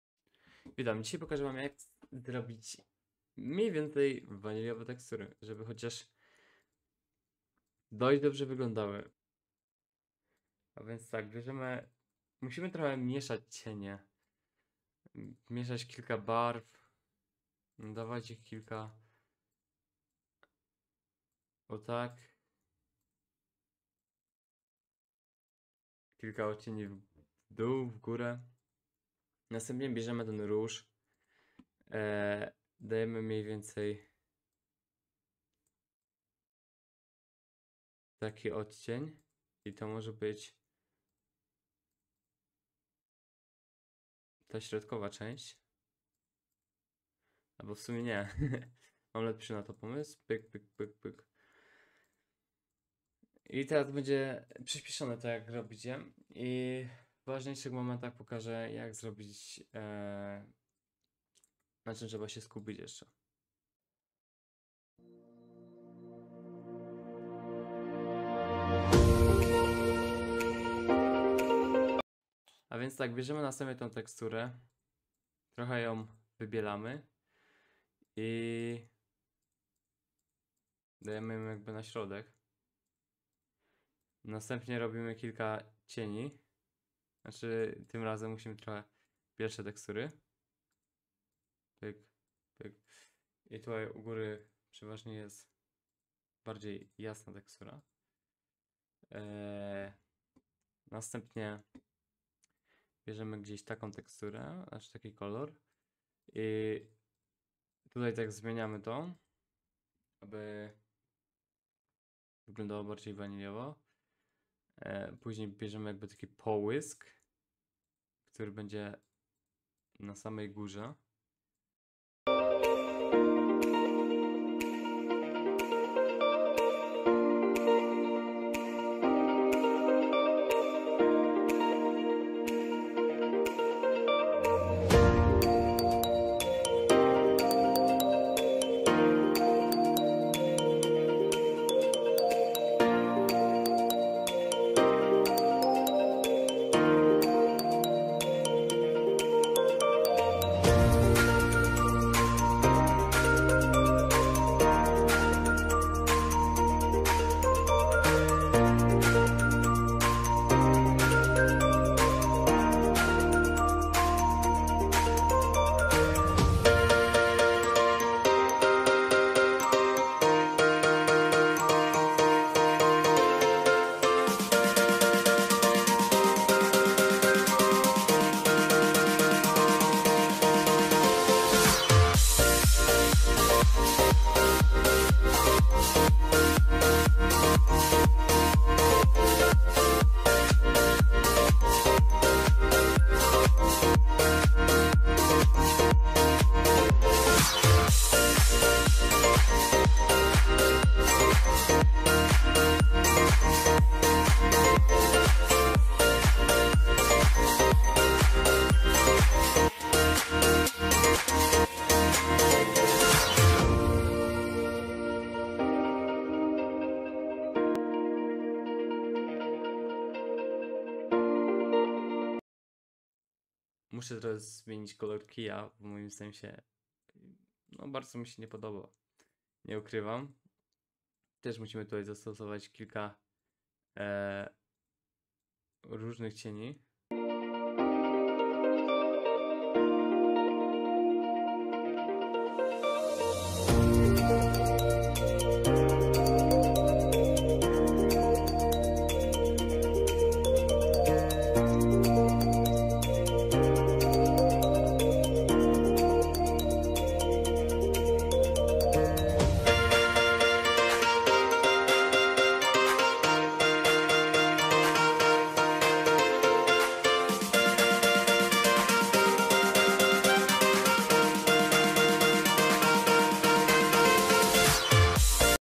Jak zrobić ładne/waniliowe textury I Poradnik Mcreator Pl
Jak zrobić w miarę waniliowe textury . Nie jestem mistrzem , ale dzięki tym radą twój mod może wyglądać lepiej . Dziękuje za obejrzenie filmu !